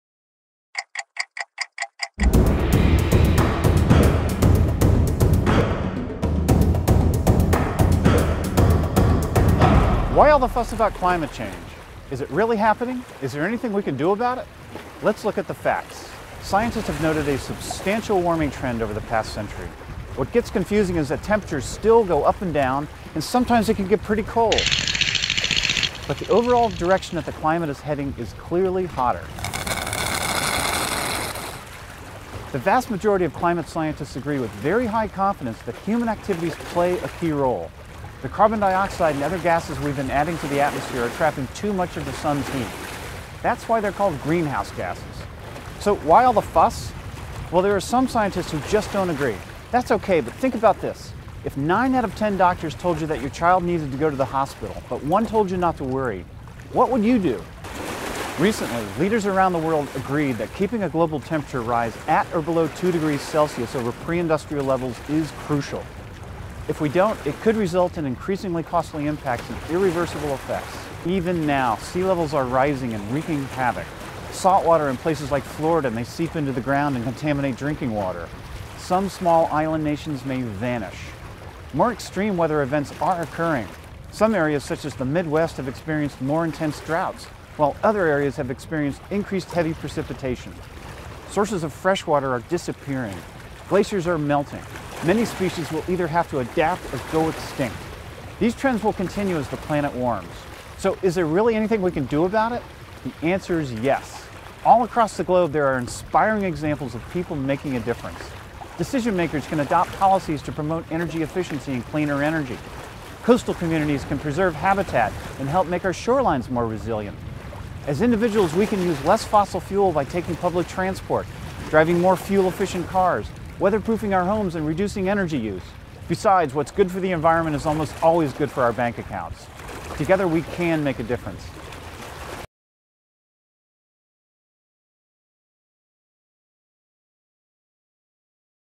Two Min on Oceans w/ Jim Toomey: The Climate Change Connection - BLUE 2014: Short Shorts Finalist
THE CLIMATE CHANGE CONNECTION
Oceans are essential for regulating the earth's climate cycle, yet substantial warming trends in the earth's atmosphere are causing dangerous changes including a rising sea level, increased extreme weather events and polar ice melts. This video connects human activities with the changing climate. Find out what’s happening and what you can do!